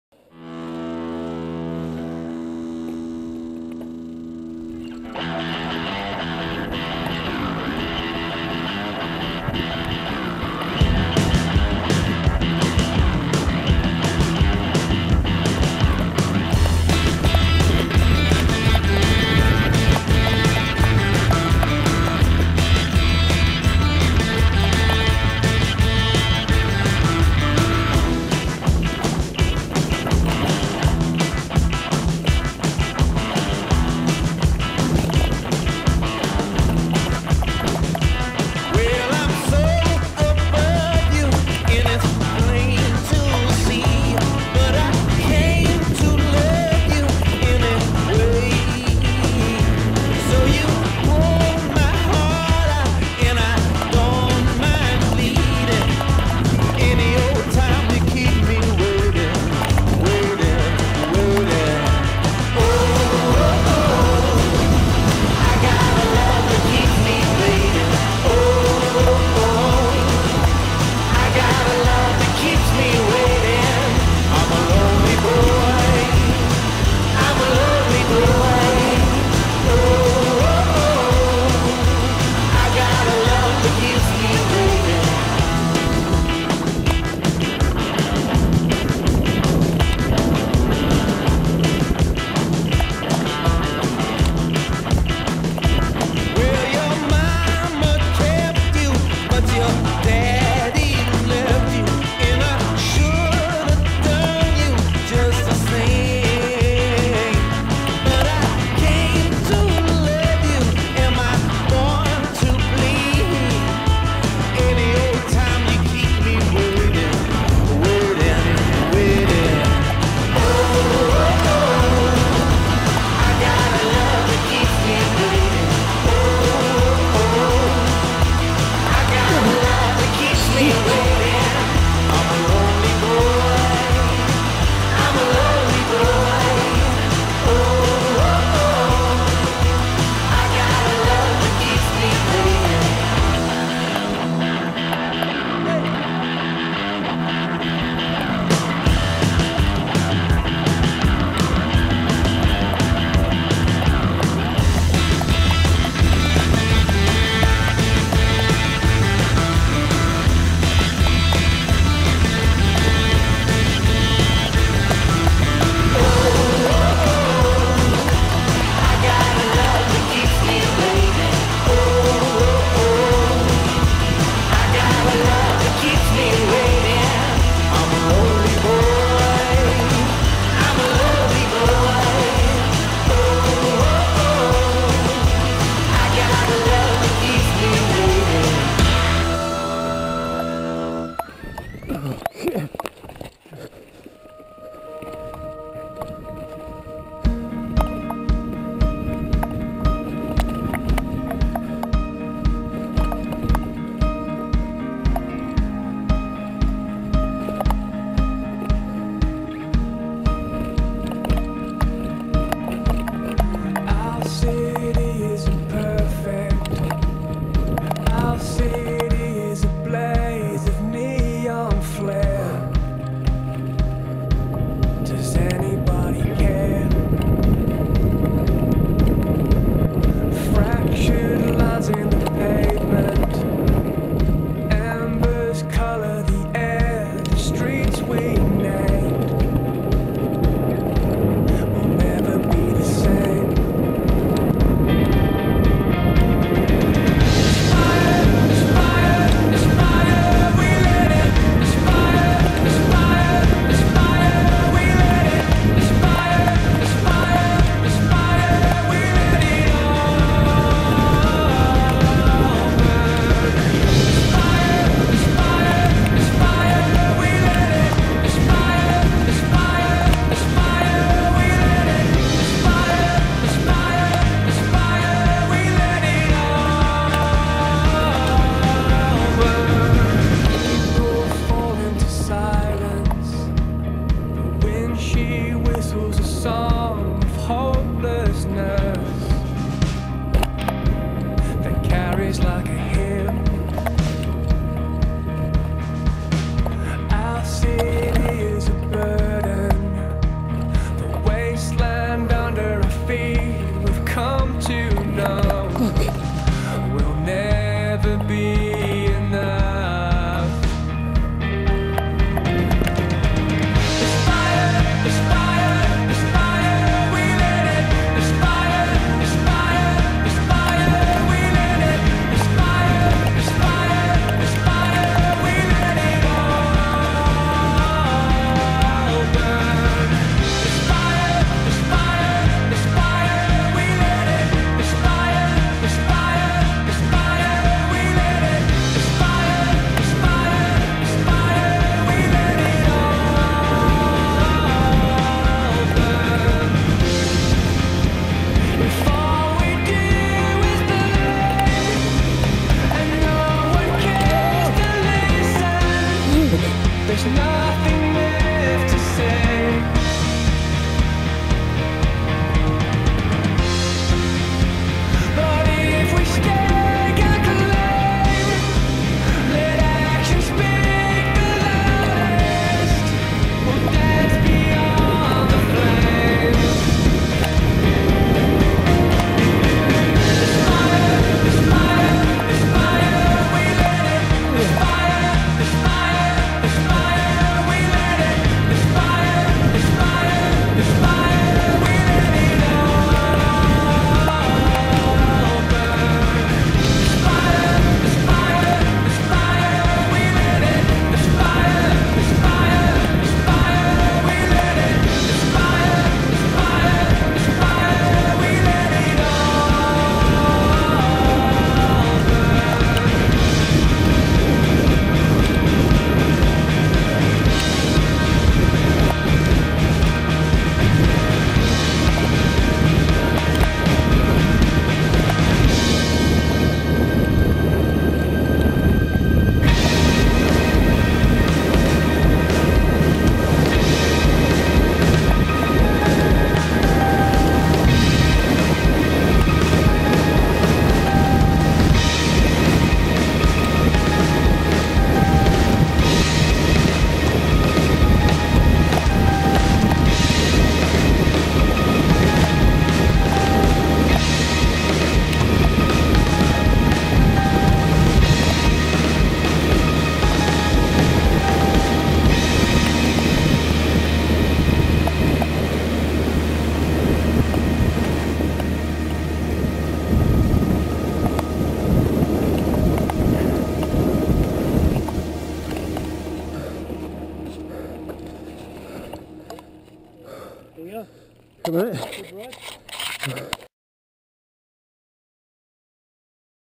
Narbethong - Epic Mud Fun
This is my last run at the end of the Apline Gravity Shuttles skills clinic with Ben at Narbethong.  Saturday was dry, but at night and all day Sunday it drizzled then rained on and off...

Bloody slippery and I have more than one "moment" on this, but by then end of the weekend I was doing stuff so much better than when I started - awesome weekend!

This track is 3 km long - by far th longest dh track I've done!